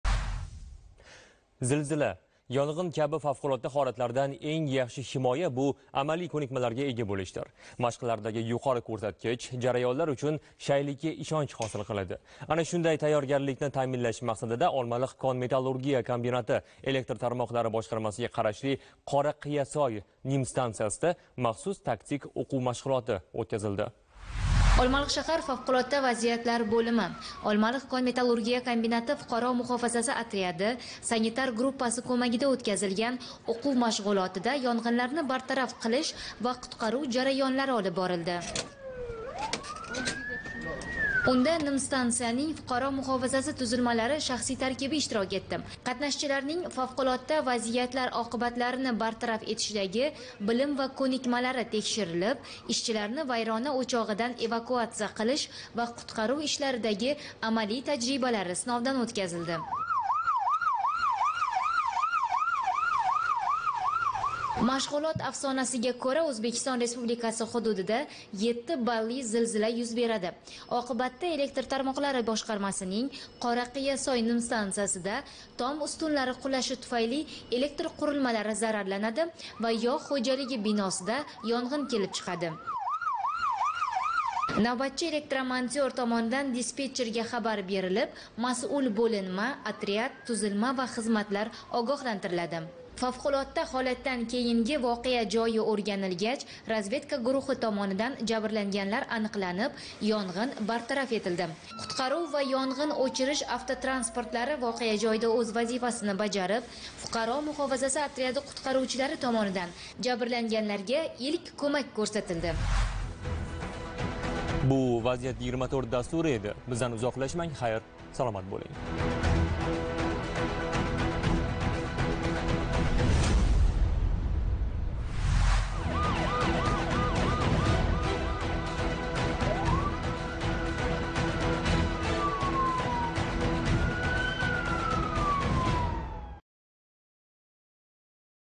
Toshkent viloyatida FVB tomonidan maxsus-taktik o‘quv mashqlari o‘tkazildi
O‘zbekiston Respublikasi Prezidentining 2023-yil 16-may kunidagi "O‘zbekiston Respublikasi aholisi va hududining seysmik xavfsizligini ta'minlash tizimini yanada takomillashtirishga oid qo‘shimcha chora-tadbirlar to‘g‘risida"gi PQ-158-son Qarori ijrosini ta’minlash maqsadida Toshkent viloyatida FVB tomonidan zilzila oqibatlarida yuzaga kelgan texnogen xususiyatli favqulodda vaziyatlarni bartaraf etish mavzusida maxsus-taktik o‘quv mashqlari o‘tkazildi.